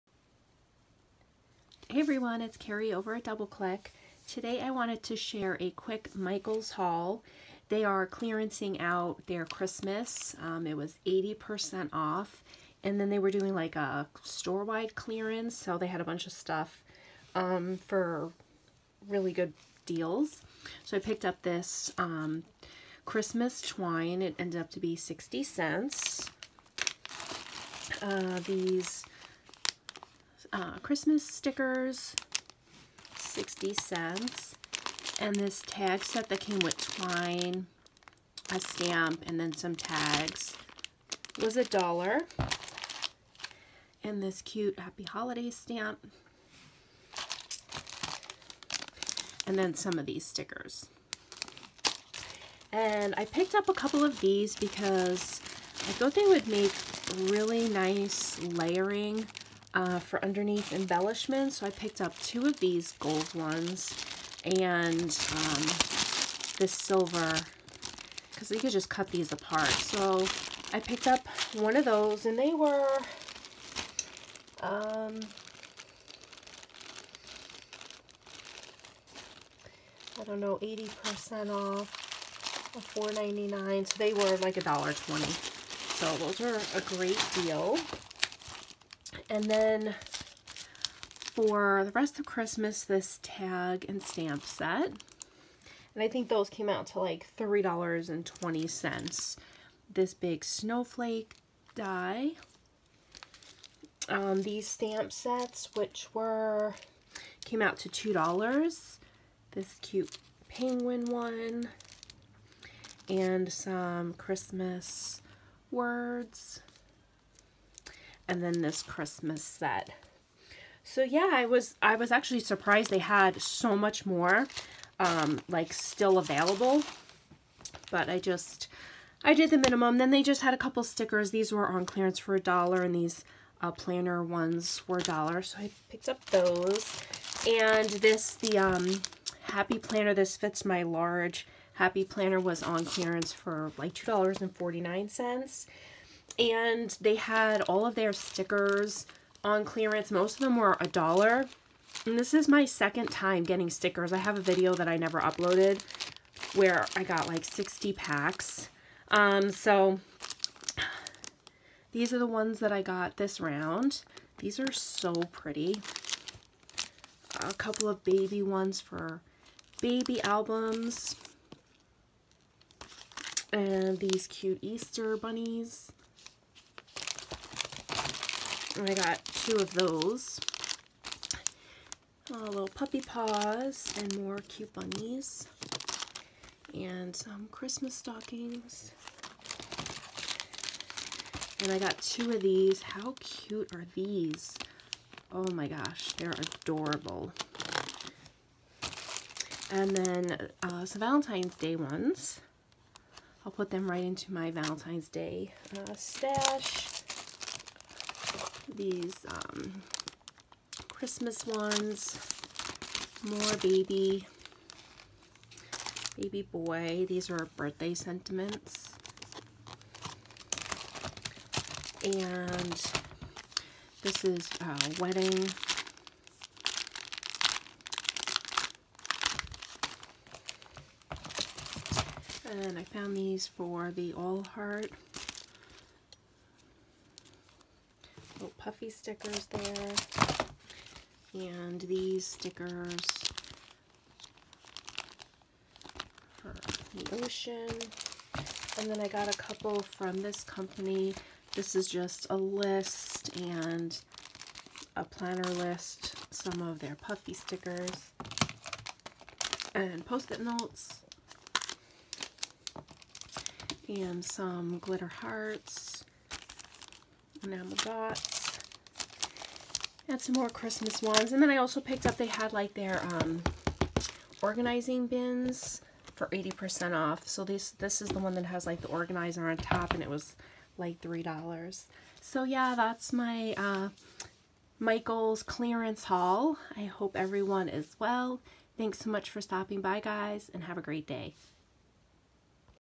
Michael’s Christmas and Sticker Clearance Paper Crafting Haul Craft Hoarder
Lots of fun paper crafting items to add to my hoard of supplies! 😂